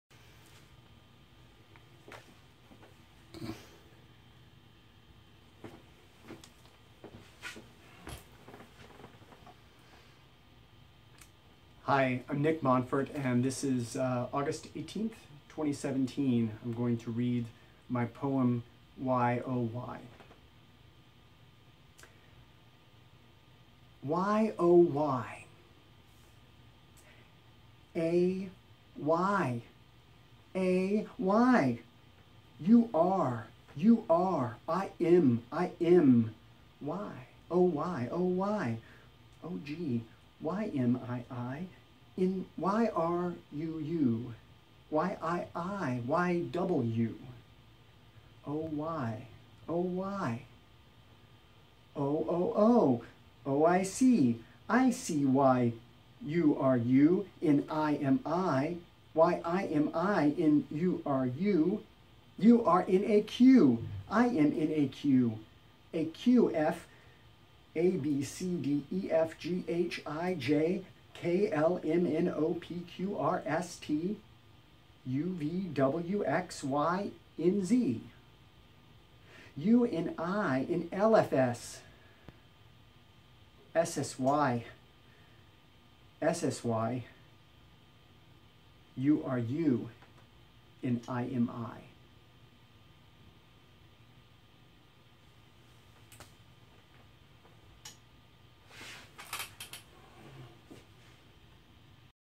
Y O Y
I read my poem “Y O Y” at the Trope Tank, MIT.